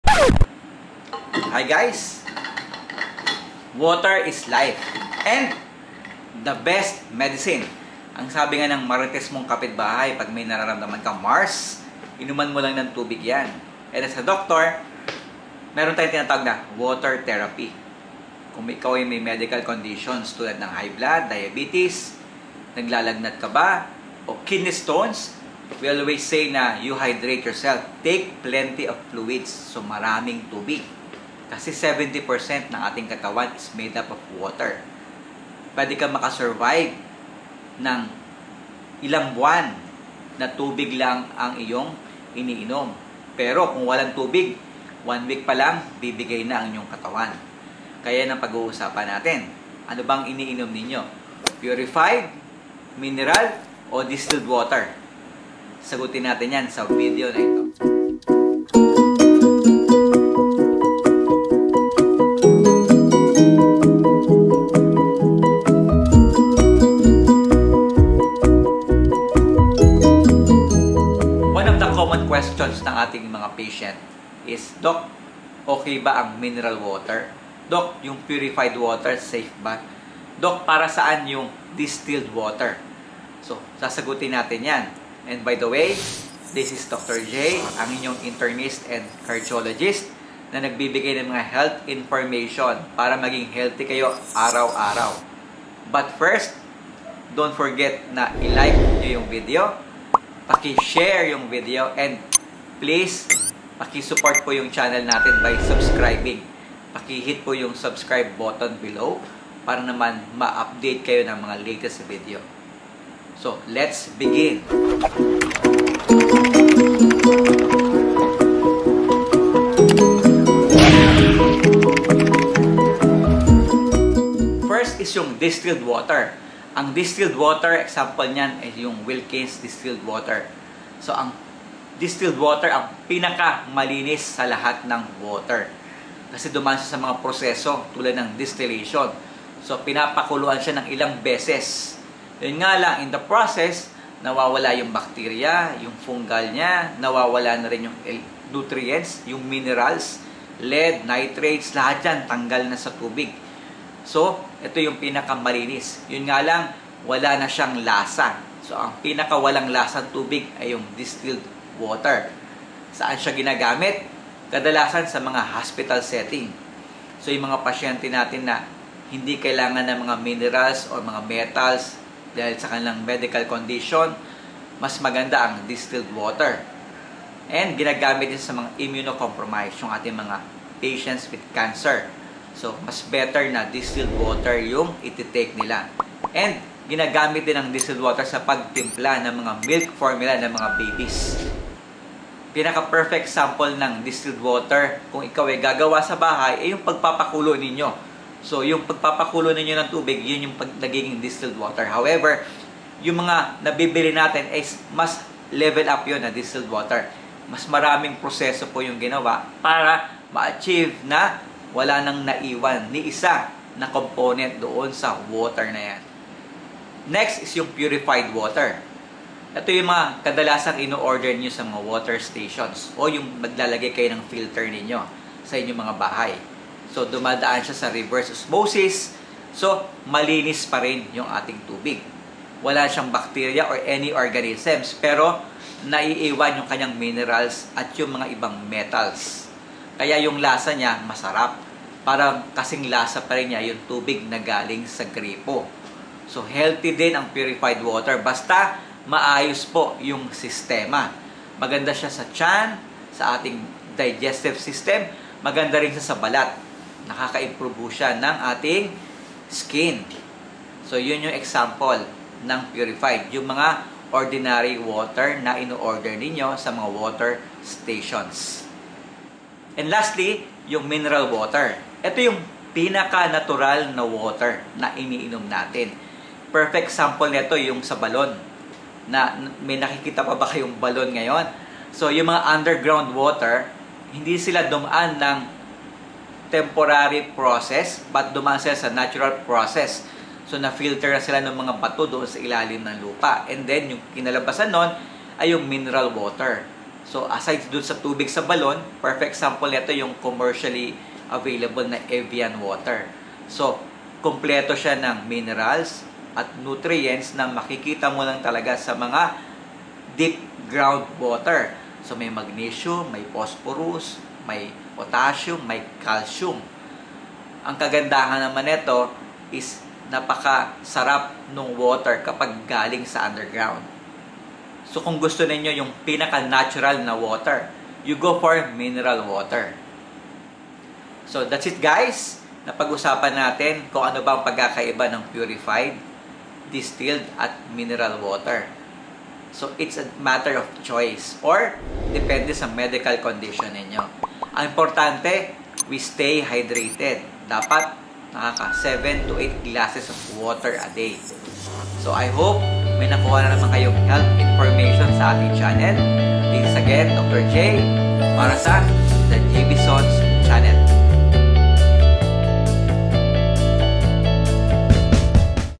Ano ang pagkakaiba ng TUBIG na iniinom mo?
Ang video na ito ay tungkol sa tubig na iniinom mo.
Purified? Mineral? Distilled water? Ano ang choice mo?
Alamin sa video na ito ni Dr. Javison -ang inyong Internist and Cardiologist sa YouTube